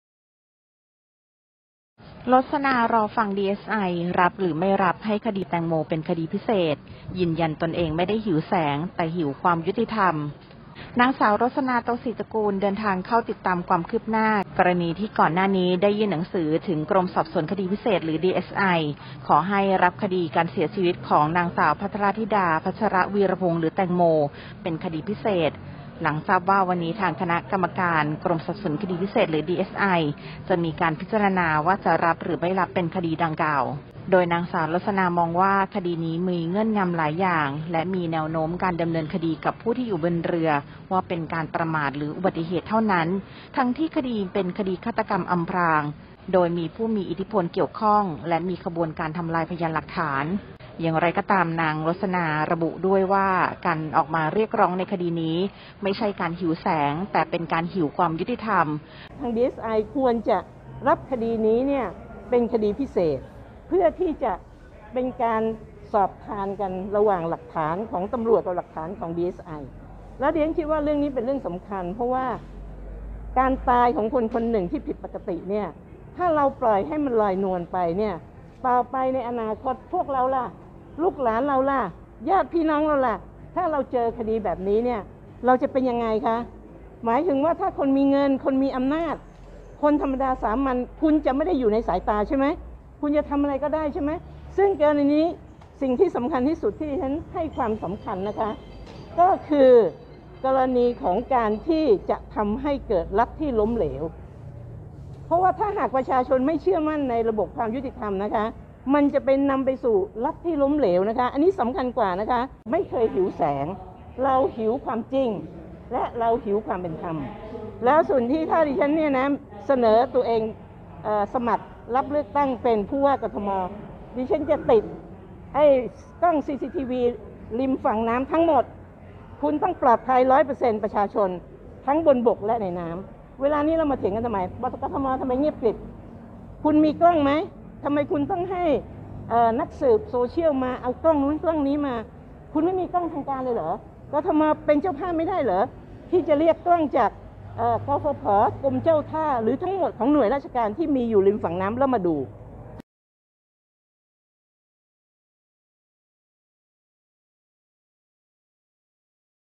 ​"รสนา" รอฟัง DSI รับหรือไม่รับ ให้คดี"แตงโม"เป็นคดีพิเศษ ยืนยันตนเองไม่ได้หิวแสง แต่หิวความยุติธรรม
น.ส.รสนา โตสิตระกูล เดินทางเข้าติดตามความคืบหน้า กรณีที่ก่อนหน้านี้ได้ยื่นหนังสือถึงกรมสอบสวนคดีพิเศษ หรือ ดีเอสไอ ขอให้รับคดีการเสียชีวิตของ น.ส.ภัทรธิดา พัชรวีระพงษ์ หรือ แตงโม เป็นคดีพิเศษ หลังทราบว่าวันนี้คณะกรรมการกรมสอบสวนคดีพิเศษ หรือดีเอสไอจะมีการพิจารณาว่าจะรับหรือไม่เป็นคดีพิเศษหรือไม่

 โดยตนเองมองว่าคดีนี้มีเงื่อนงำหลายอย่างและมีแนวโน้มการดำเนินคดีกับผู้ที่อยู่บนเรือ ว่าเป็นการประมาทหรืออุบัติเหตุ ทั้งที่คดีอาจป็นคดีฆาตกรรมอำพราง โดยมีผู้มีอิทธิพลเกี่ยวข้อง และมีขบวนการทำลายพยานหลักฐาน

อย่างไรก็ตาม นางรสนา ยืนยันว่าที่ออกมาเรียกร้องคดีนี้ ไม่ใช่การหิวแสง แต่หิวความยุติธรรม โดยที่ผ่านมาติดตามเรื่องความเป็นธรรมมาตลอดและตนเองมีแสงในตัวเองอยู่แล้ว โดยขอไม่พูดถึงประเด็นครอบครัวของแตงโม โดยเฉพาะแม่ของแตงโมที่ไม่ประสงค์ให้เป็นคดีพิเศษ แต่ที่ออกมาเรียกร้องเพราะอยากให้เป็นบรรทัดฐาน ย้ำไม่ได้เกาะกระแสคดีแน่นอน